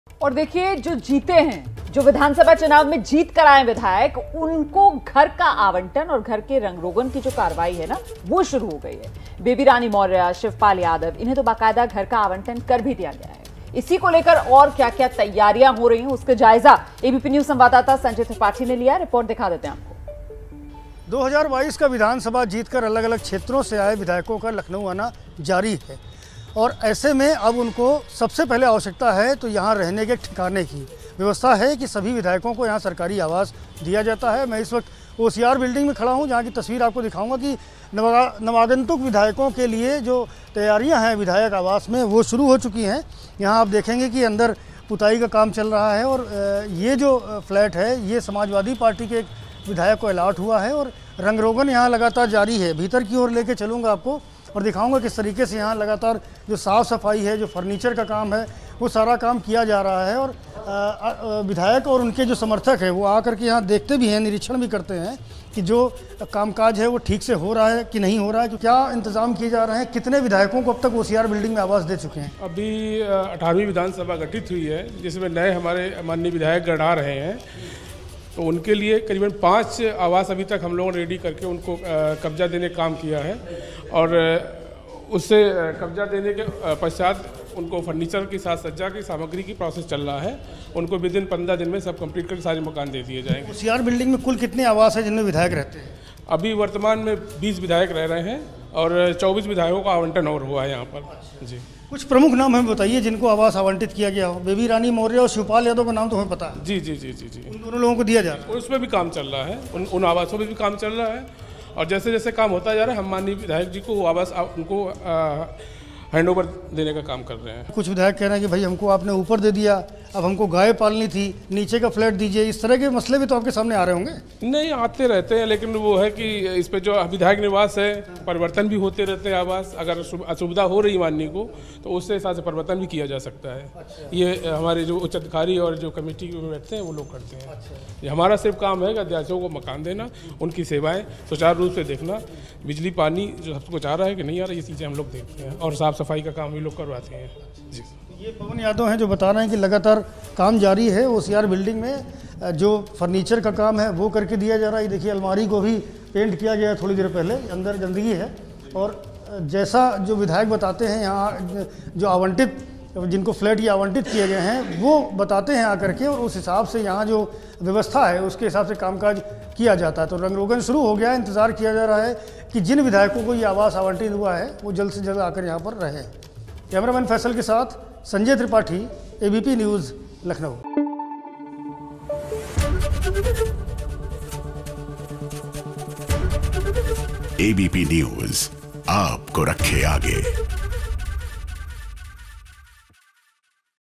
UP में निर्वाचित हुए विधायकों को आंवटित किए जा रहे हैं घर, देखिए ये रिपोर्ट
UP में निर्वाचित हुए विधायकों को आंवटित किए जा रहे हैं घर...शिवपाल यादव और बेबी रानी मौर्य को घर हुआ आंवटित...मरम्मत का कार्य जारी.